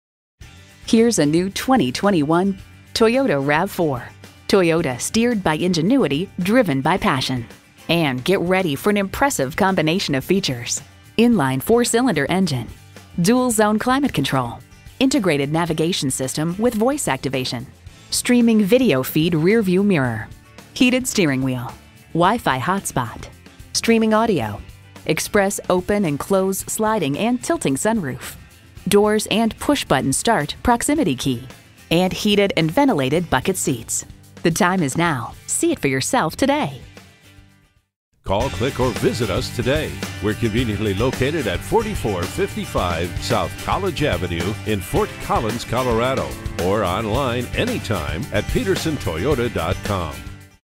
New 2021 Toyota RAV4 Fort Collins Loveland, CO M39070 - SOLD
Year: 2021
Make: Toyota
Model: RAV4
Trim: Hybrid Limited
Engine: 4 2.5 L
Transmission: Variable
Color: Midnight Black Metallic
Interior: Ash
Stock #: M39070
VIN: 2T3D6RFV2MW016578

Address:
4455 S. College Ave